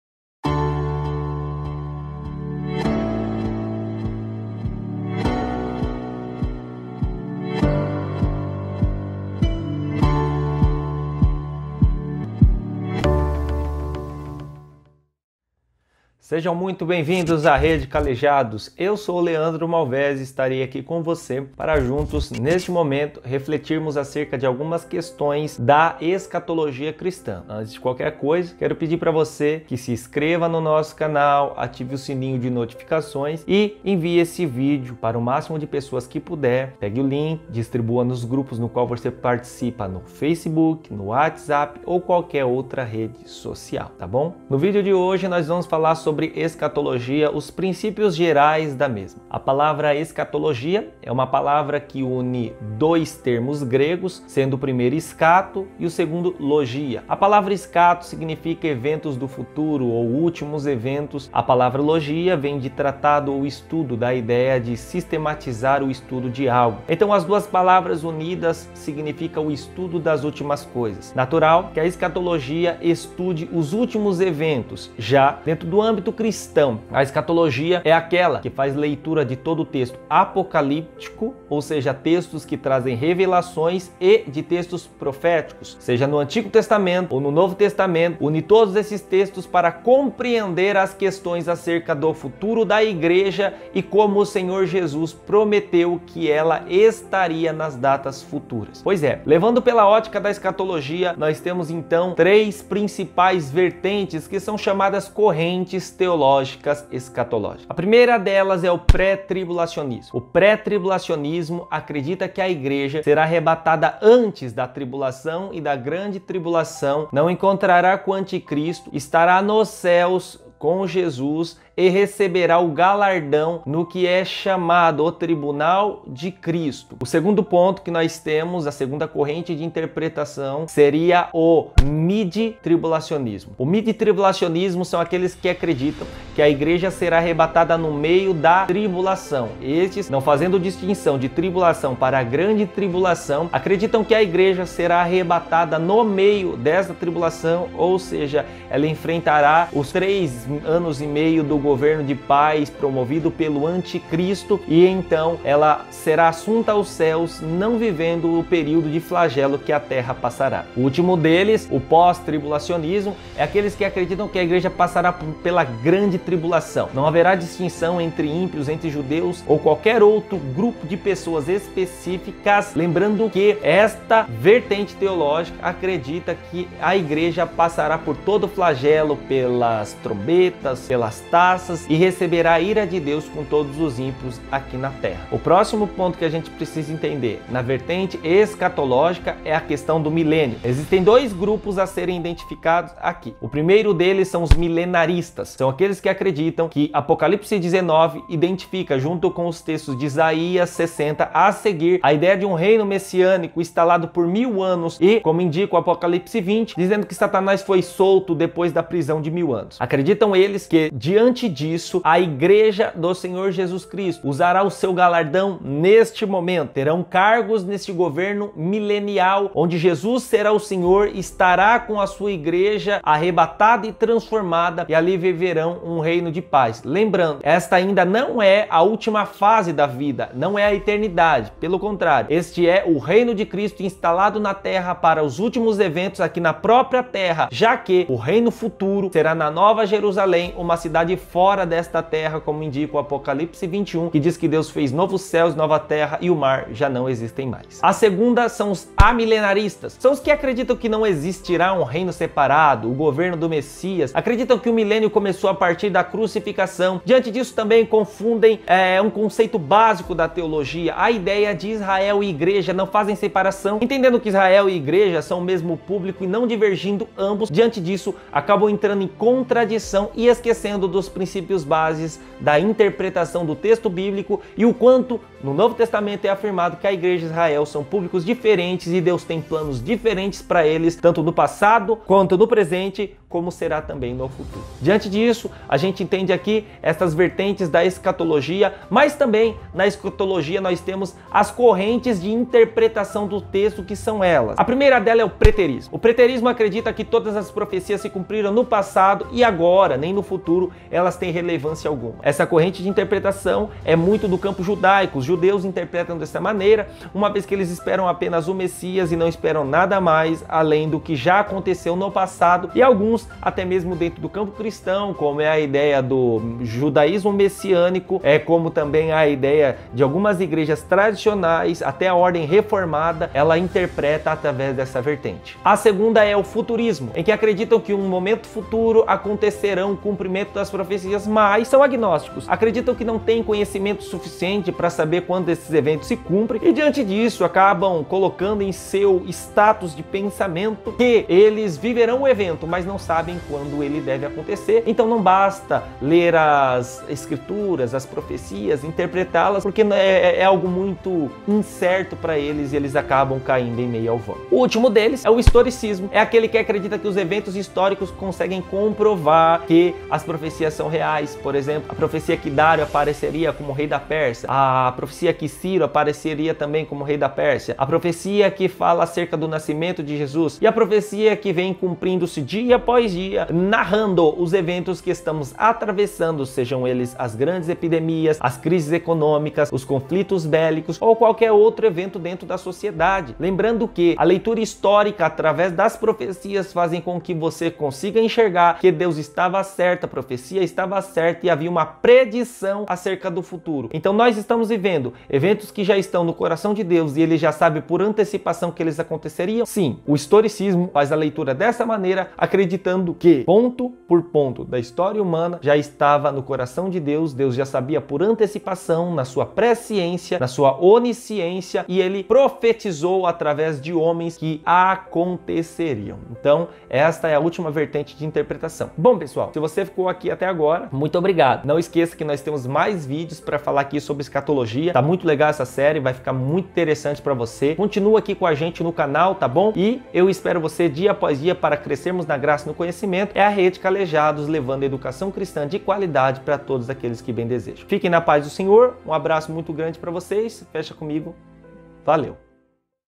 Os princípios gerais da Escatologia Série Os últimos dias #0001
Nessa série estudaremos sobre os últimos eventos da humanidade através de uma leitura bíblica que seguirá a corrente de interpretação "PRE-TRIBULACIONISTA", então, pedimos que você siga cada episódio e não perca nada.

Pegue papel, caneta e vamos estudar juntos!

Parceiros: @QueridaDani , @valdemirpmoreira1981 , @generacionpentecostal.